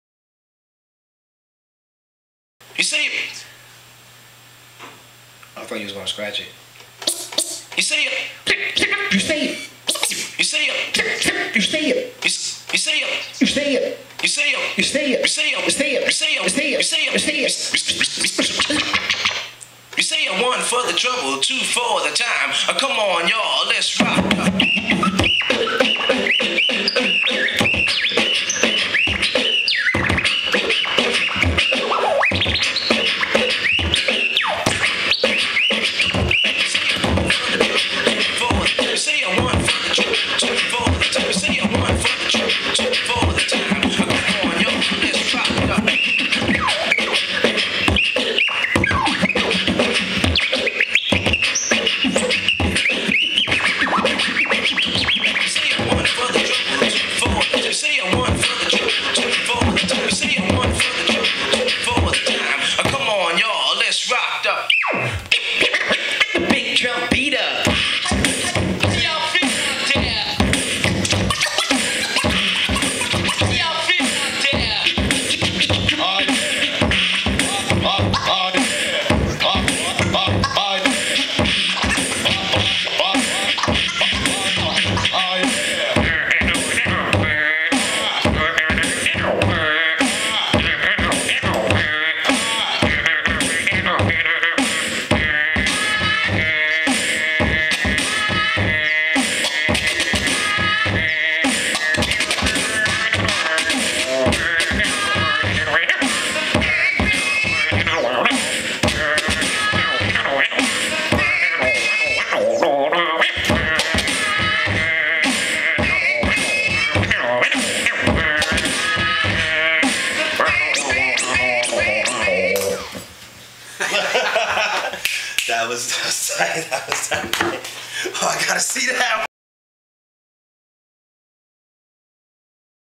Skratch Bastid & Scratch - "Spoonie Gee/Run-DMC" Beatbox/DJ
Live footage of Skratch Bastid and Scratch (of The Roots fame) running through ideas for their upcoming show @ Revival Bar on Saturday, November 14th.

Skratch Bastid Presents...
SCRATCH, SKRATCH, SCRATCH!
Saturday, November 14th, 2009
Revival Bar, 783 College St, Toronto

Scratch, Skratch Bastid & DJ Starting From Scratch
Opening set by mymanhenri (92bpm)